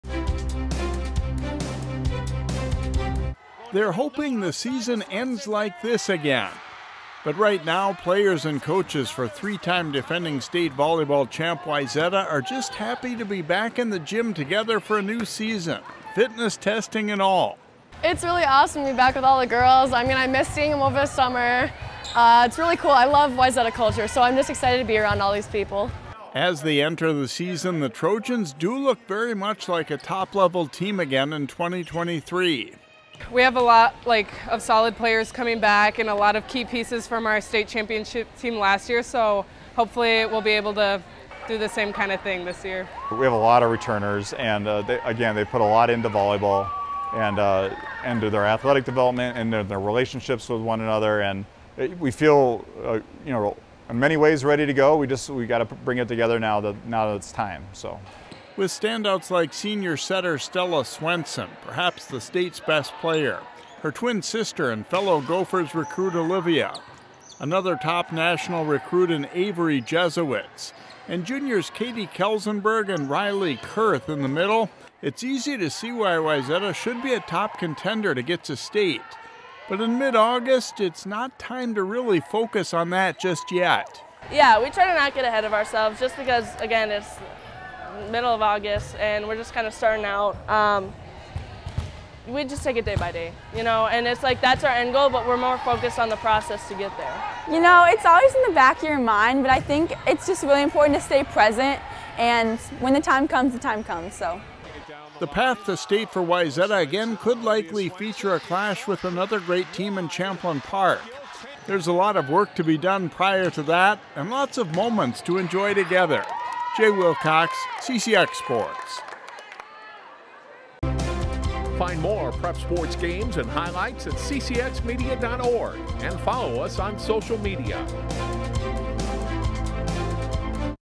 Wayzata Volleyball Starts Practice to Pursue Another Title
The Wayzata volleyball team is aiming high again as practices get started for the 2023 season. The Trojans have won three consecutive state titles (there was no tournament in 2020 due to the pandemic).

They lost just two starters from last year’s title team, and have some of the state’s top players in setter Stella Swenson and outside hitters Avery Jesewitz and Olivia Swenson. Getting back to state to defend their title is the end goal but the Trojans are focusing on the process of getting better each day.